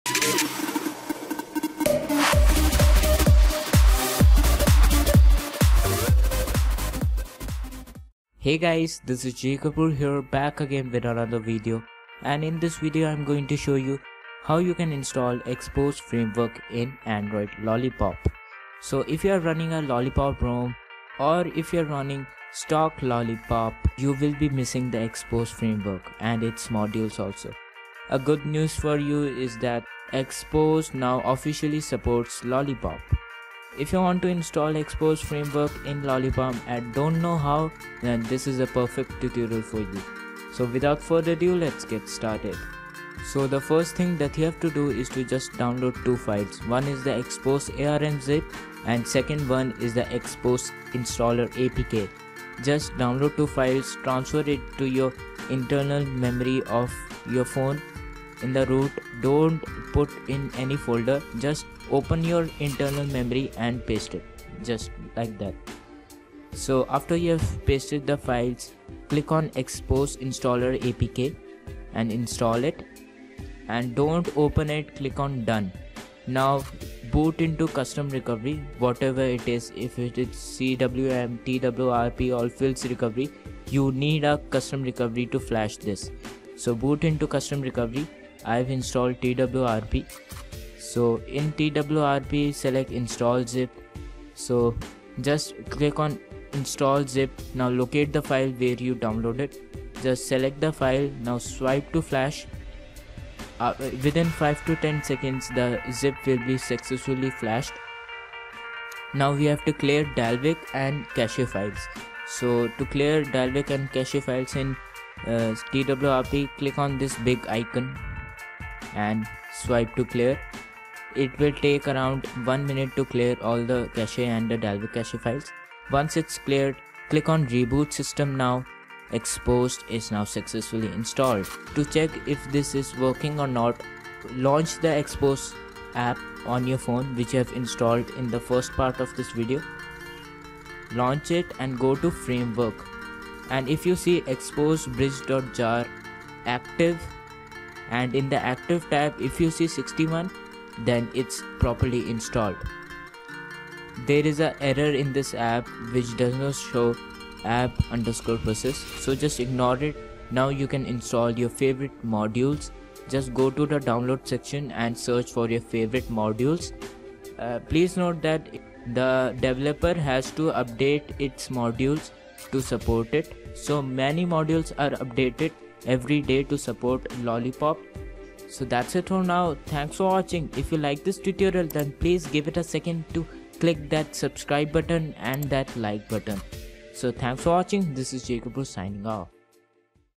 How to install Xposed framework in Lollipop
It's a detailed tep by step guide to install Xposed Framework in android Lollipop 5.0.Xposed framework now officialy supports Android Lollipop.Now you can enjoy all your favourite Xposed Modules like Gravitybox(Lp).This Method will NOT WORK in Samsung ROMS (Touchwiz).